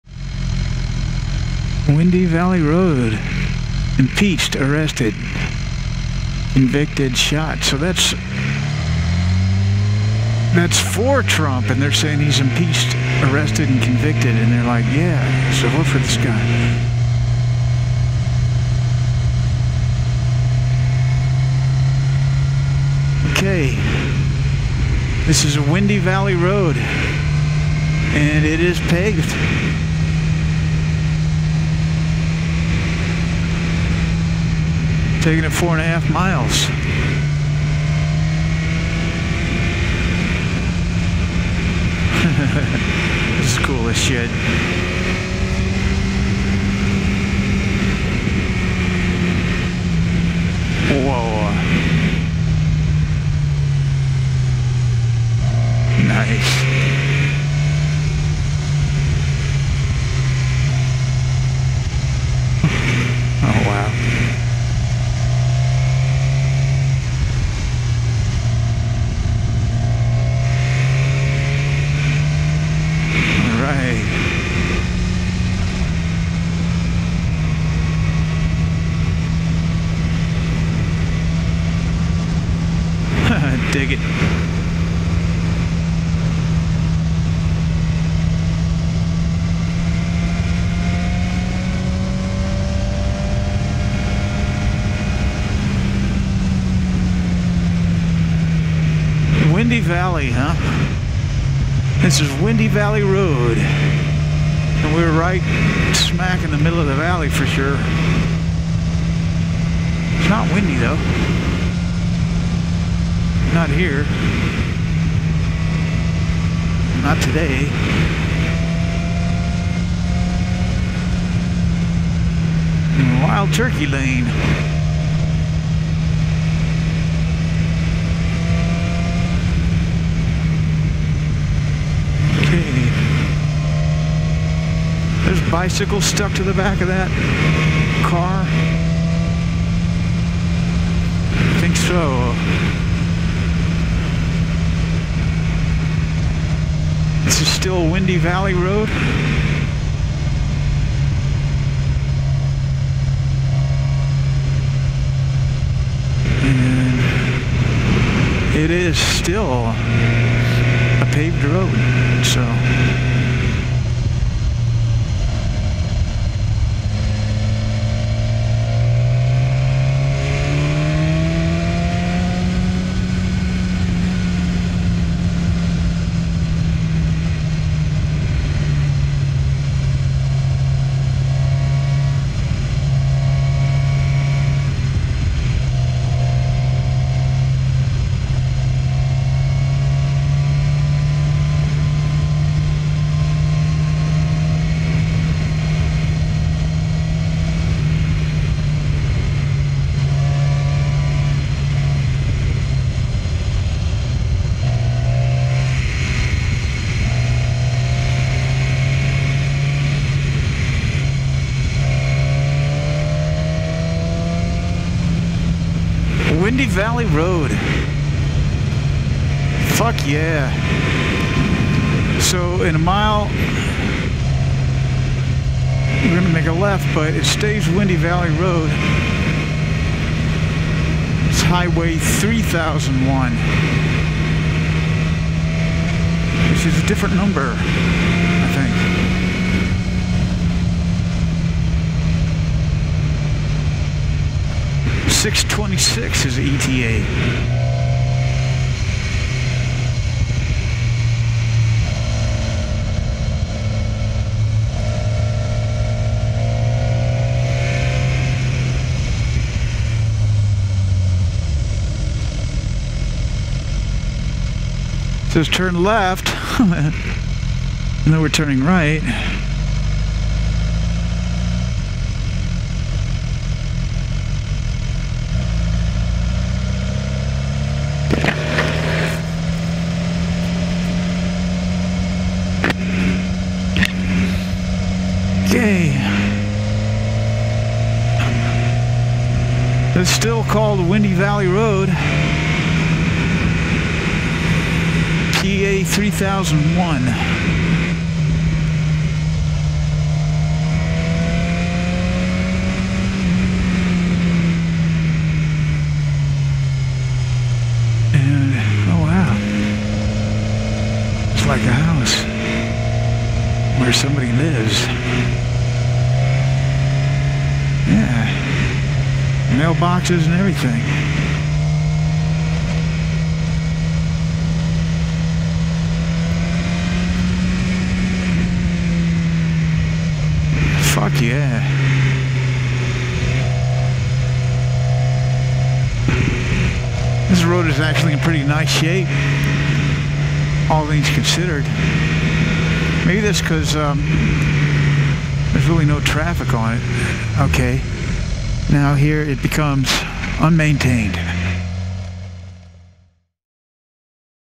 Seizing the Day on Windy Valley Road, Forkston Twp PA
I wanted a way to get from Forkston to the town of Lopez, other than PA-87 west to PA-487 south.  On the map was a tempting but dubious option:  Windy Valley Road to Bellasylva Road, covering nine miles along Mehoopany Creek.  Neither road came up on Google Street View, but I decided to go for it.  Windy Valley turned out to be a fully-paved, top-tier riding road -- Bellasylva not so much.